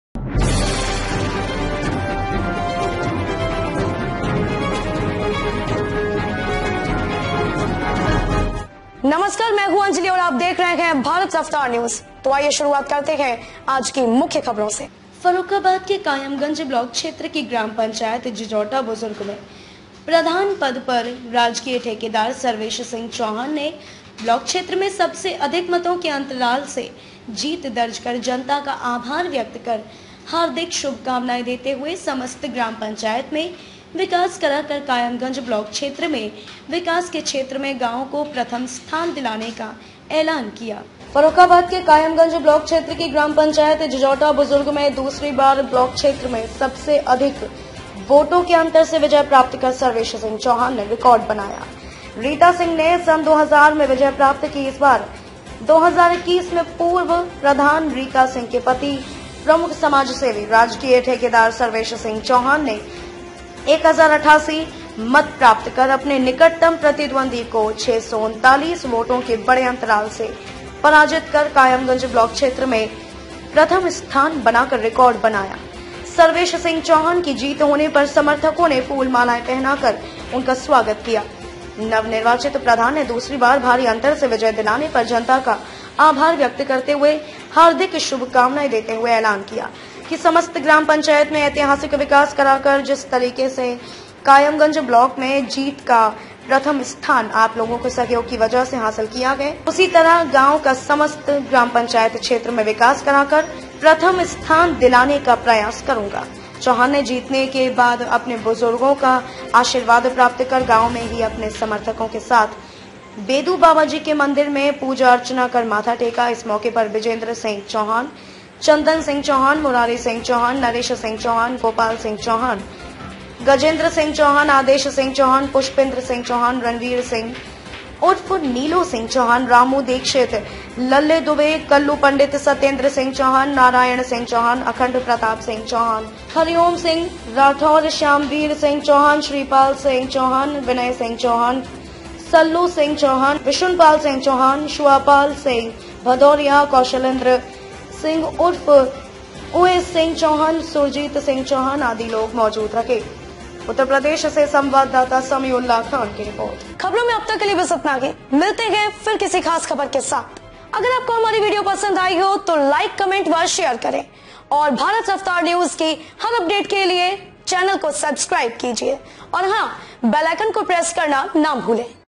फर्रुखाबाद यूपी : ठेकेदार सर्वेश सिंह चौहान ने दूसरी बार सबसे अधिक वोटों के अंतर से विजय प्राप्त की
फर्रुखाबाद यूपी
कायमगंज ब्लॉक क्षेत्र में राजकीय ठेकेदार सर्वेश सिंह चौहान ने दूसरी बार सबसे अधिक वोटों के अंतर से विजय प्राप्त की